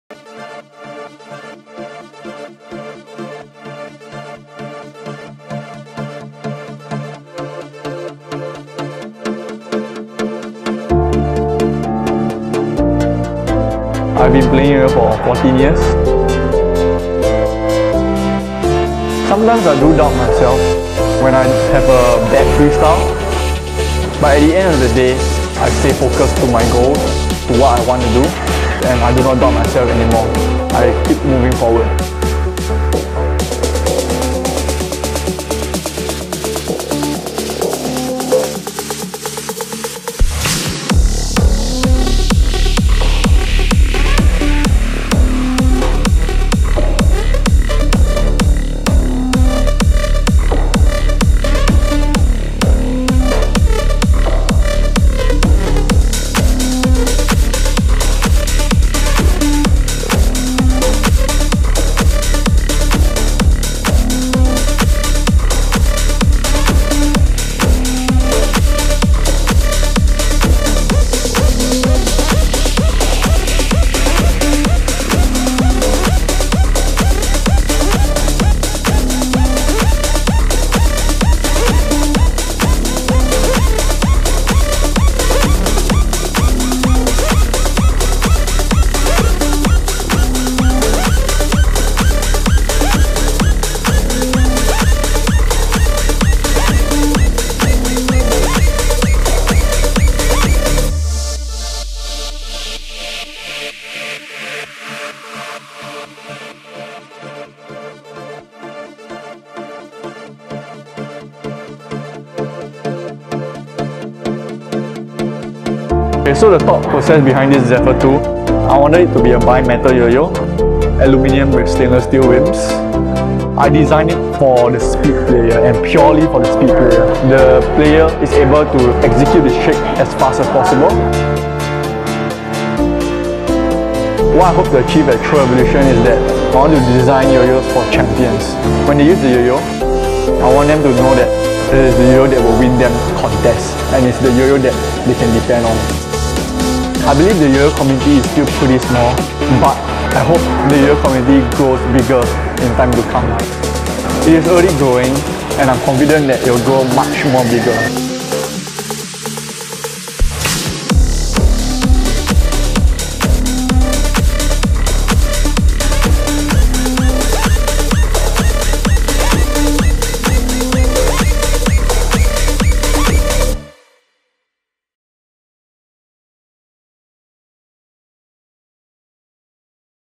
The Story of ZEPHYR II ft. Marcus Koh
A short video to promote the release of the ZEPHYR ll.
Do check out other amazing YOYOs that Throwrevolution has to offer